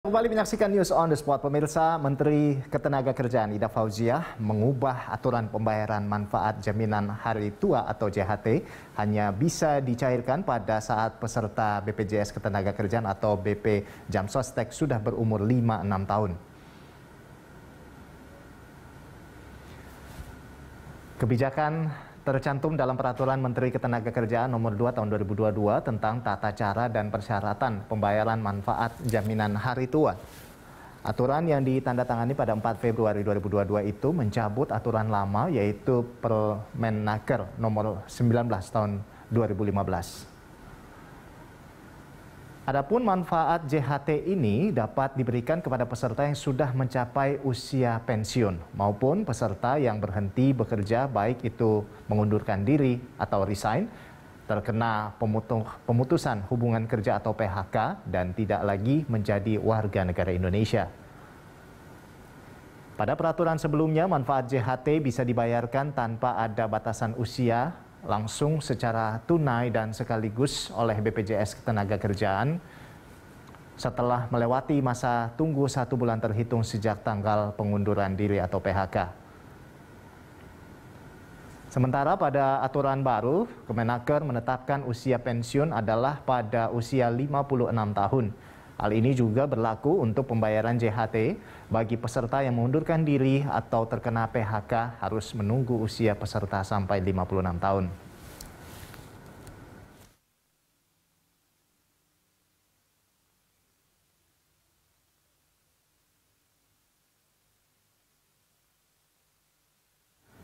Ingin Cairkan JHT dari BPJS Ketenagakerjaan? Tunggu 56 Tahun
Menteri Ketenagakerjaan Ida Fauziyah mengubah aturan pembayaran manfaat jaminan hari tua atau (JHT) hanya bisa dicairkan pada saat peserta BPJS Ketenagakerjaan atau BP Jamsostek sudah berumur 56 tahun. Hal ini juga berlaku untuk pembayaran JHT bagi peserta yang mengundurkan diri atau terkena PHK harus menunggu usia peserta sampai 56 tahun baru dana bisa cair.